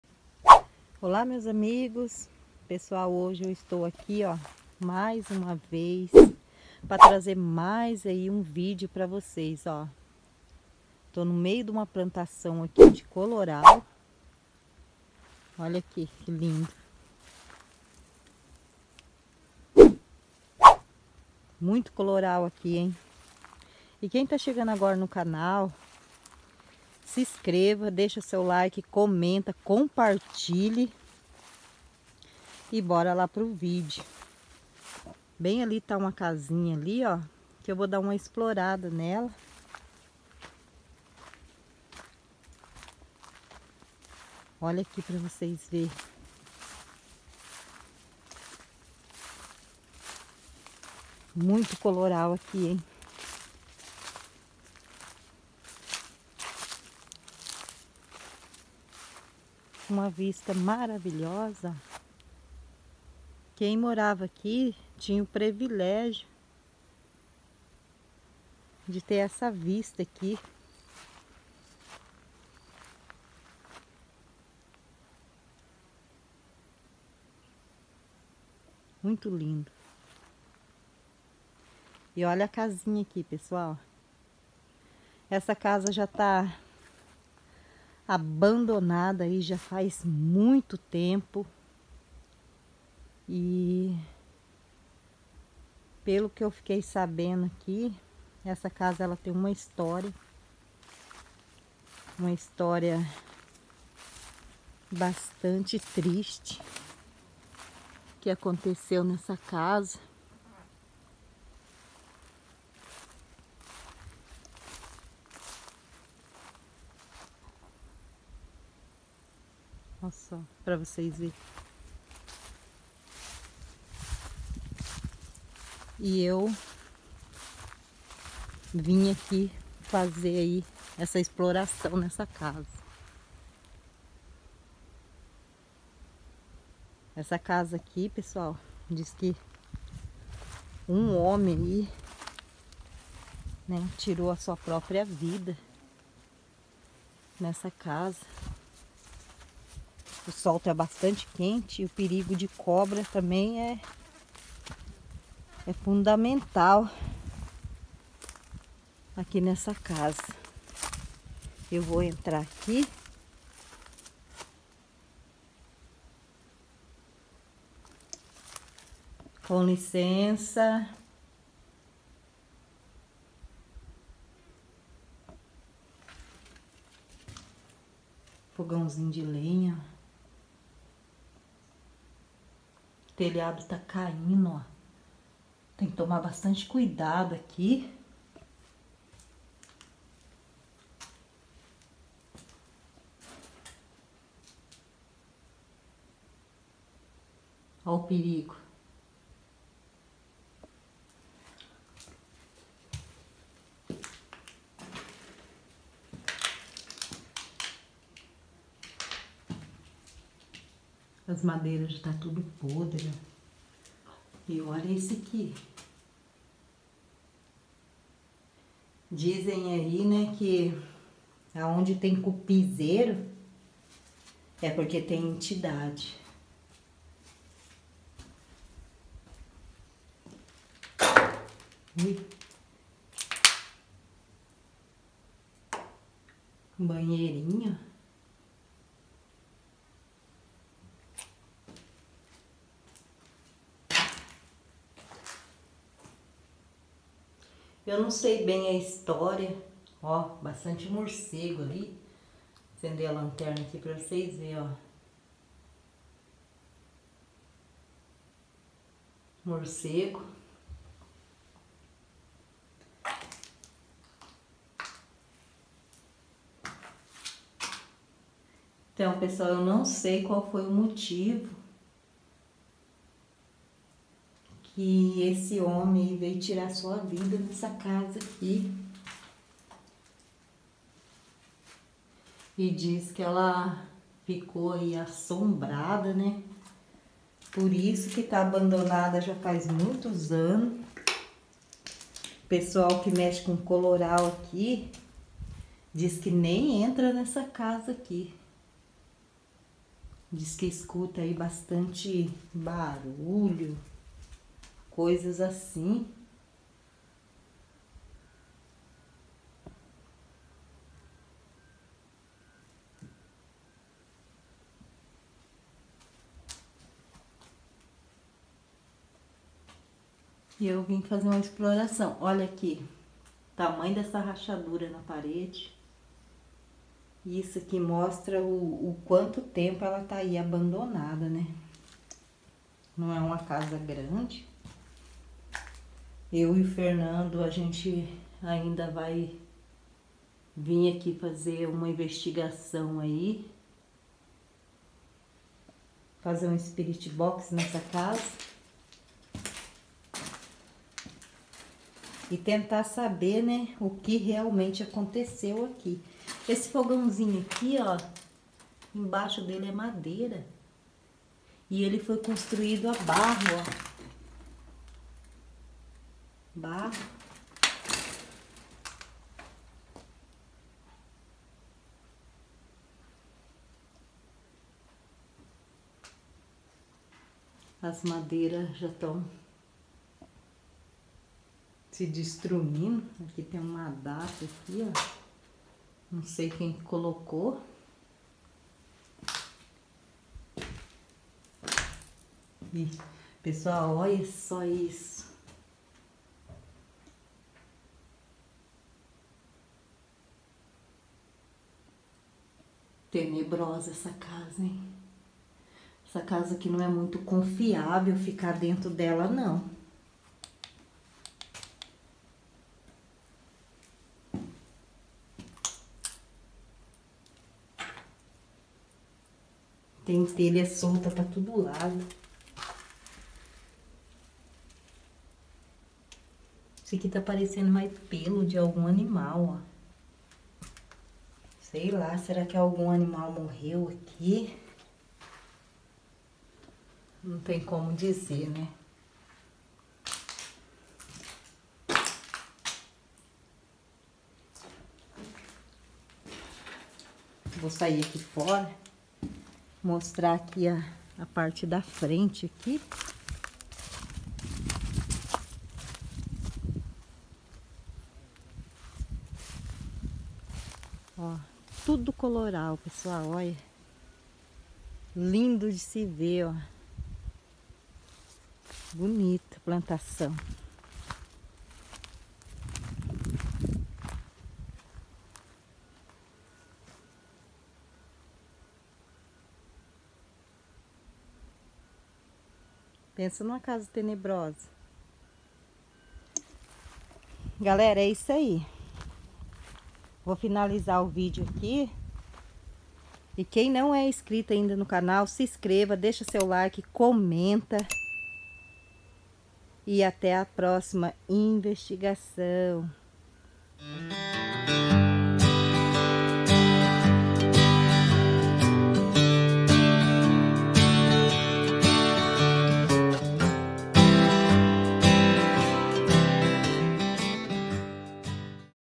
Veja oque tinha dentro dessa casa abandonada era assustador
Fui investigar uma casa abandonada e quando entrei nela vi algo terrível perto do forro assista até o final do vídeo obrigada 🙏